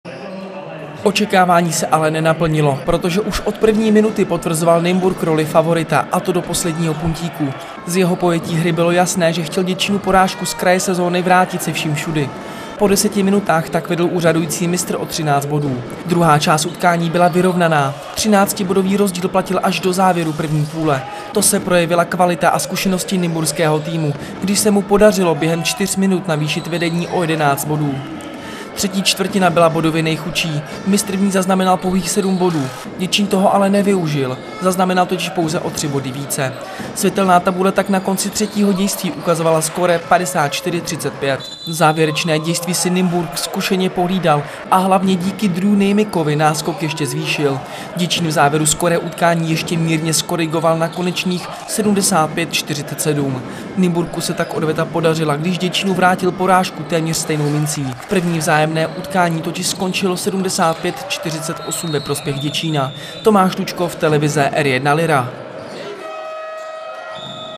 TV R1 Lyra: V Nymburku se Děčínu nedaří
Reportáž TV R1 Lyra.Po domácím vítězství nad Ostravou zajížděl ambiciózní Děčín na půdu mistra z Nymburku. Děčínským basketbalistům se letos jako jediným podařilo nymburského obra porazit.